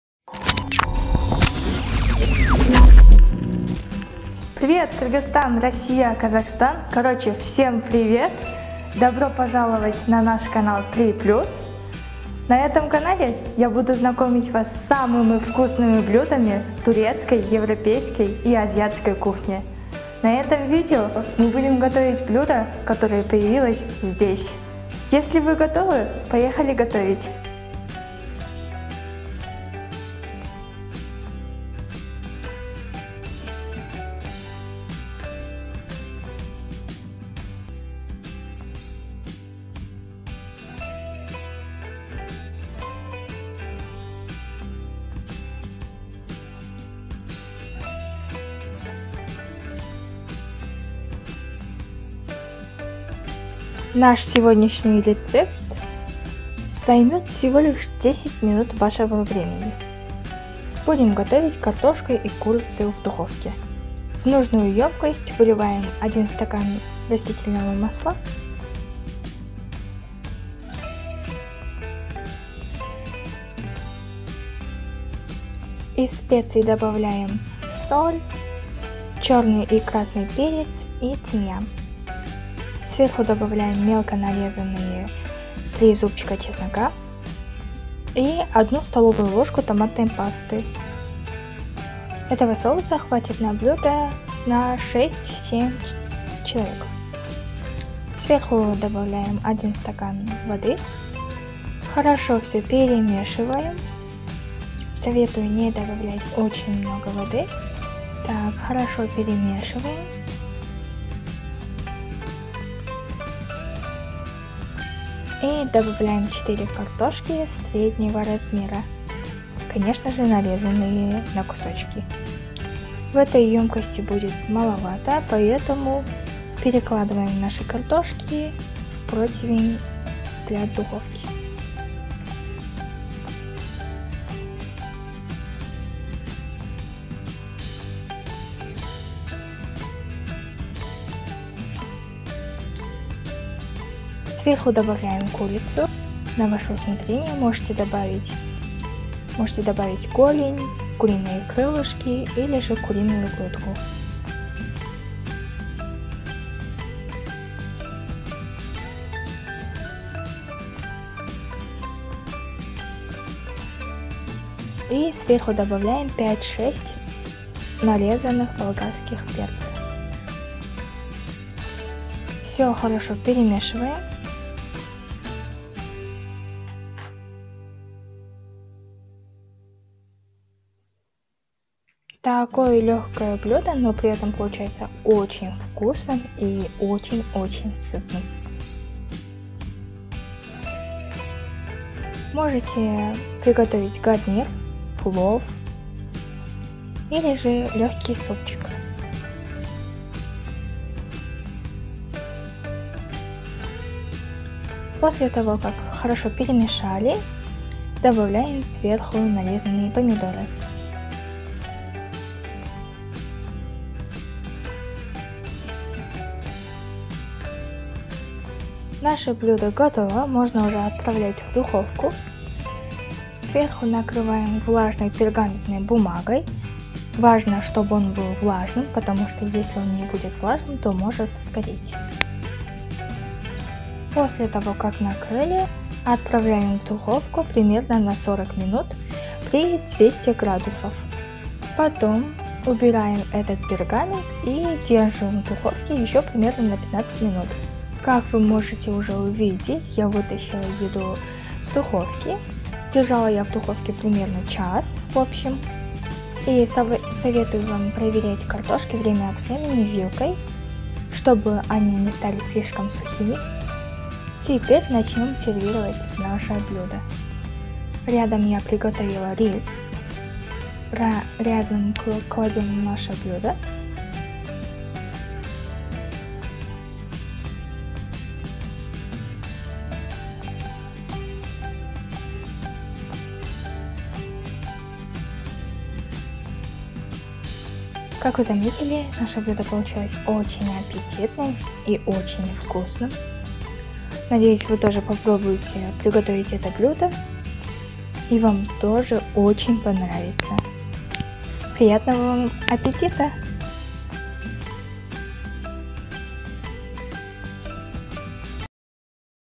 Невероятная запеченная курочка с овощами - Картошка и курица в духовке с соусом
3plus Tv
Ингредиенты:
5 голеней
1 куриная грудка (можно любую курицу)
5,6 длинных зеленых перца
1 помидор
1 красный перец
соль, перец, перец чили, перец молотый, тимьян
1 чашка масла
2 зубчика чеснока
Половина столовой ложки томатной пасты
4 средних картофеля
Ingredients:
5 shins
1 chicken breast (any chicken is possible)
5.6 long green peppers
1 tomato
1 red pepper
salt, pepper, chili pepper, ground pepper, thyme
1 cup butter
2 cloves of garlic
Half a tablespoon of tomato paste
4 medium potatoes

Alıntı : Seval Mutfakta